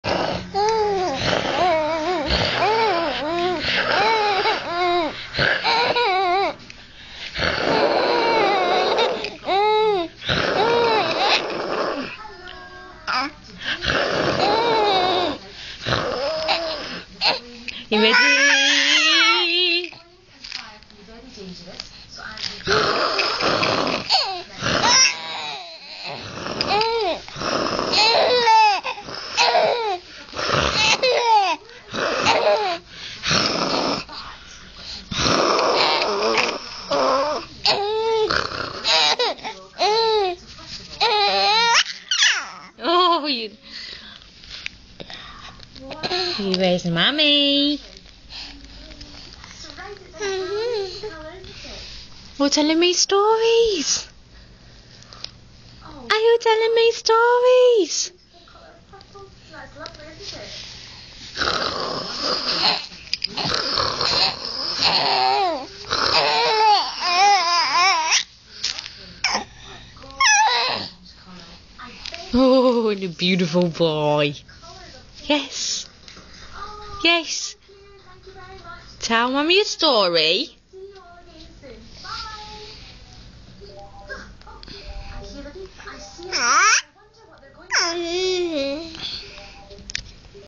archie thomas 22-4 laughing
this is for archies mummy. special littel man, my sisters son and my own nephew. littel bundel of joy!!